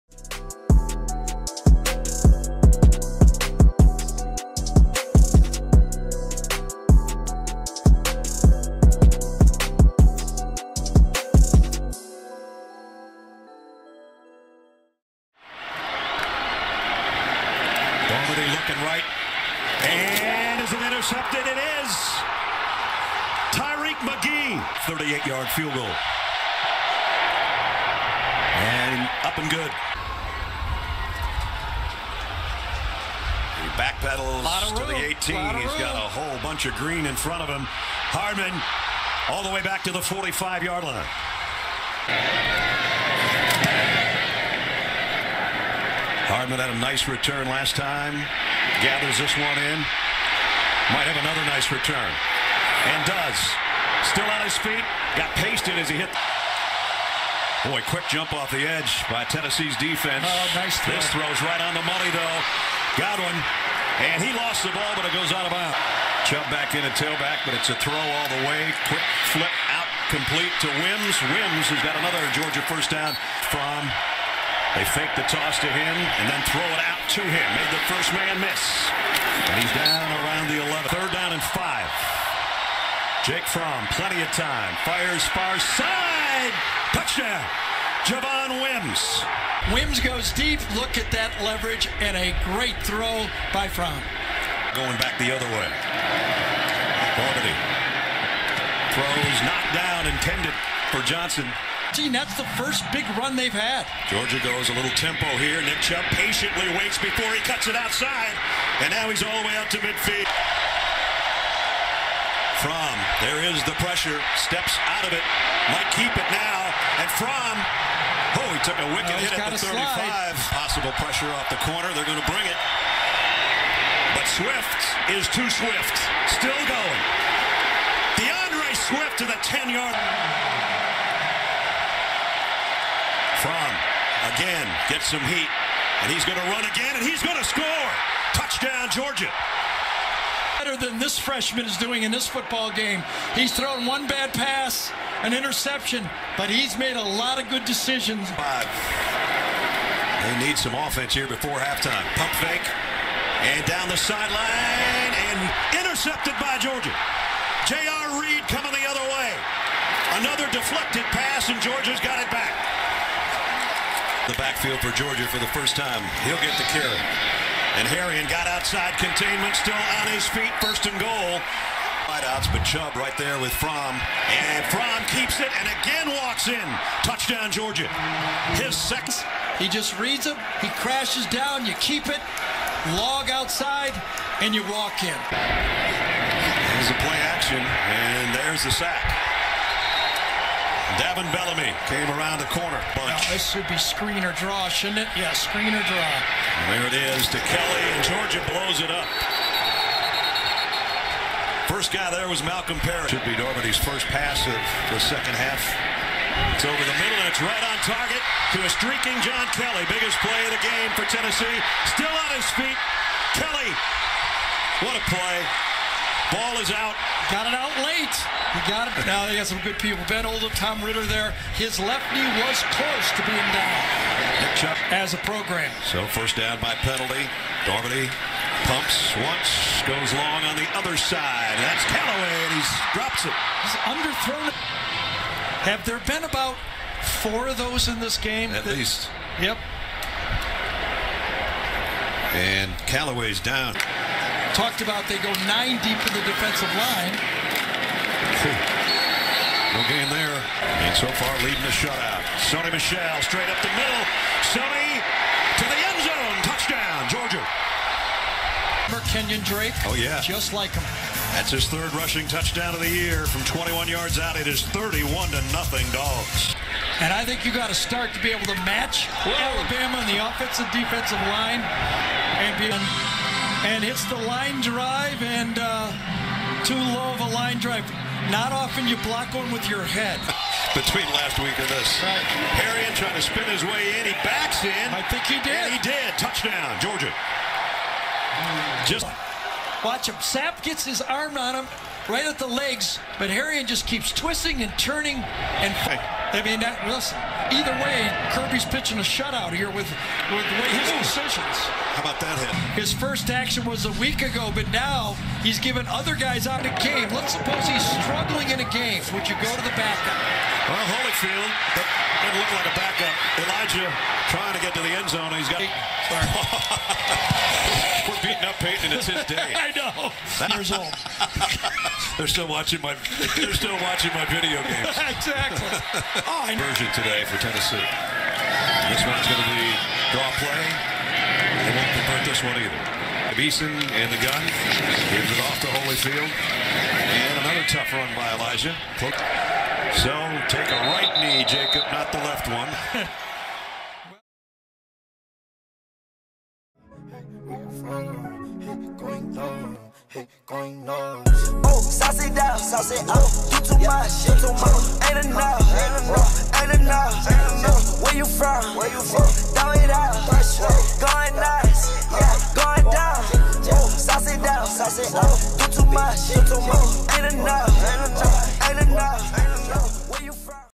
#7 Georgia Highlights Vs. Tennessee 2017 | CFB Week 5 | College Football Highlights 2017
* Suggest video ideas in the comments below *